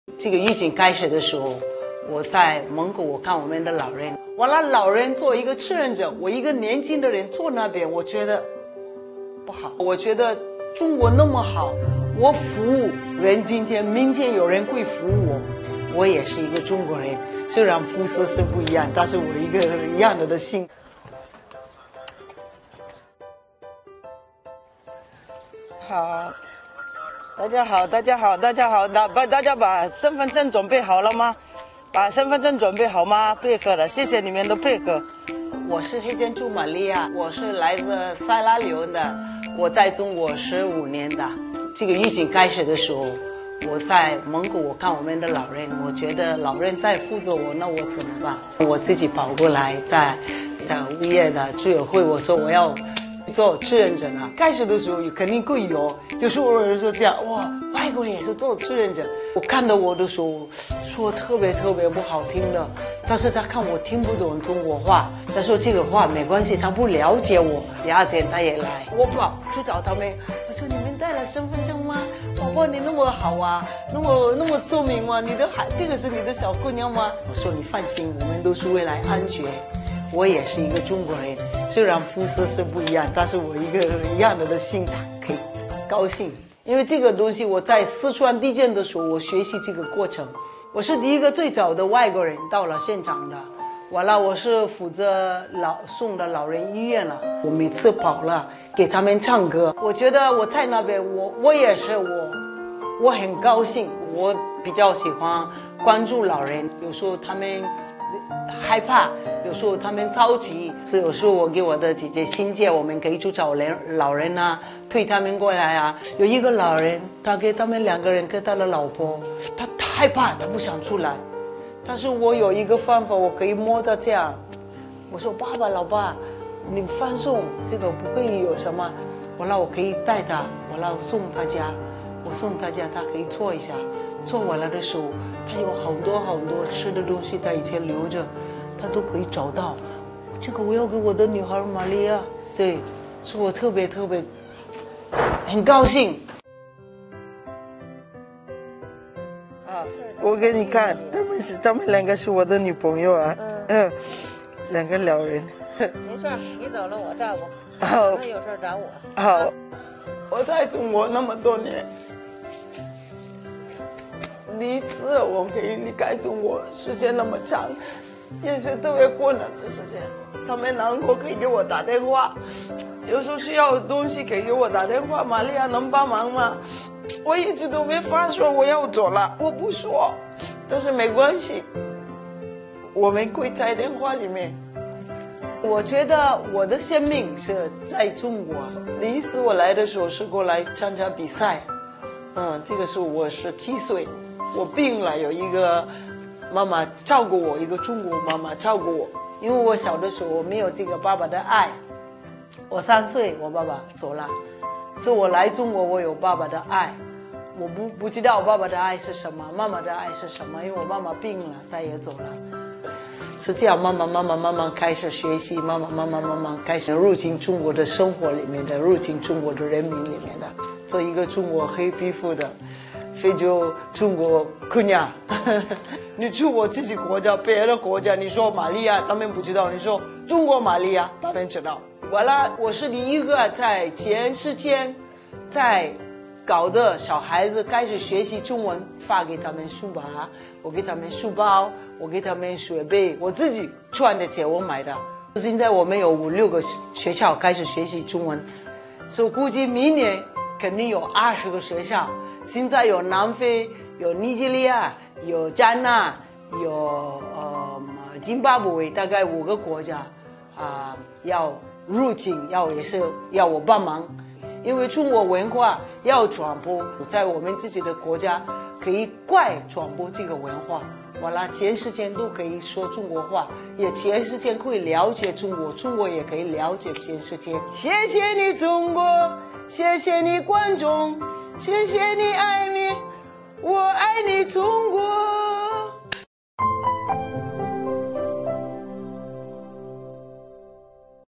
世界小姐在北京做抗疫志愿者：自称是非洲“中国姑娘”
在北京市朝阳区东风乡东润枫景社区东风南小区的核酸检测点位，有一个靓丽的身影，她就是来自塞拉利昂的玛丽亚。她2007年来到中国参加比赛，因《星光大道》被中国观众熟知。她还曾获得世界小姐双料冠军，被人们称作“黑珍珠”。如今，玛丽亚已在中国生活15年，将这里视为自己的第一故乡。疫情发生后，她第一时间联系社区成为志愿者，坚守一线。她还一直致力于在多个国家传播中国文化，希望全世界都了解中国。